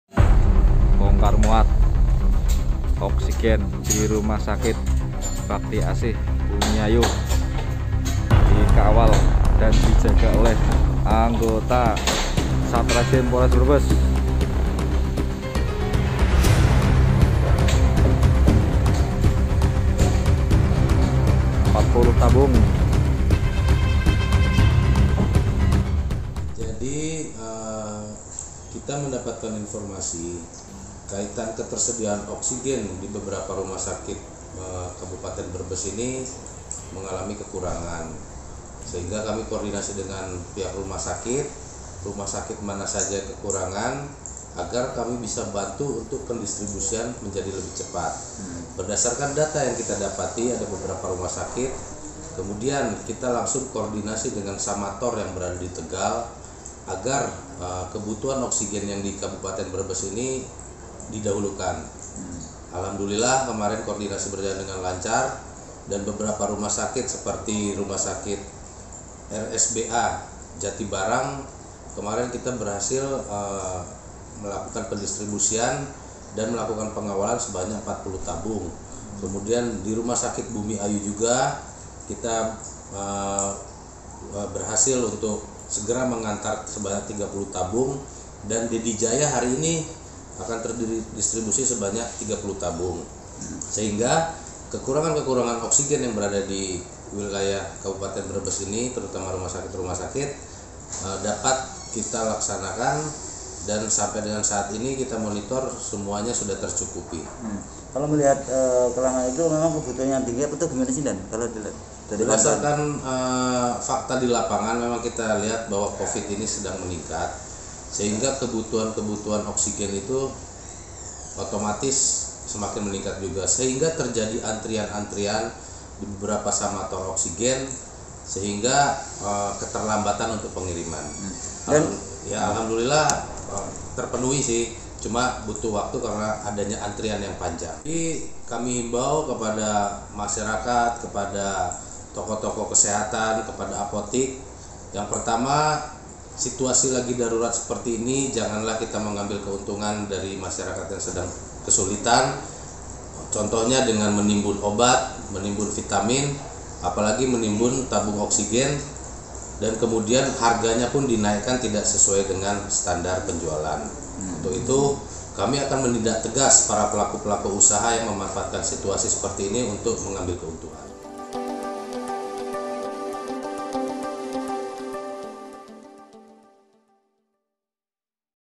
Polisi Brebes Ancam Tindak Pihak yang Menaikkan Harga Oksigen dan Obat-obatan
BREBES – Kepala Satuan Reserse Kriminal (Kasatreskrim) Polres Brebes AKP Hadi Handoko mengingatkan kepada para penjual oksigen dan obat-obatan untuk tidak menimbun dan menaikkan harga. Apalagi di tengah permintaan yang melonjak saat pandemi.

“Kami tekankan khususnya kepada retail-retail, jangan sampai ada yang bermain atau coba menimbun dan menaikkan harga. Karena kami pantau langsung di lapangan,” kata Hadi Handoko, Kamis, 8 Juli 2021.

Ia menambahkan, pantauan di lapangan juga dilakukan di tingkat distributor. Polisi, kata Hadi, tak segan menindak tegas apabila ditemukan penjual oksigen ataupun obat-obatan yang menimbun hingga menaikkan harga.